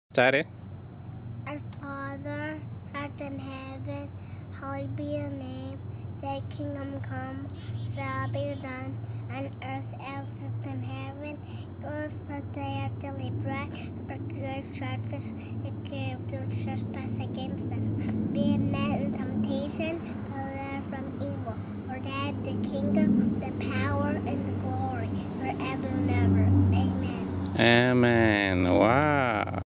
3-Year-Old Girl Praying
You can't go wrong with a little prayer!! Especially if it's coming from a three-year-old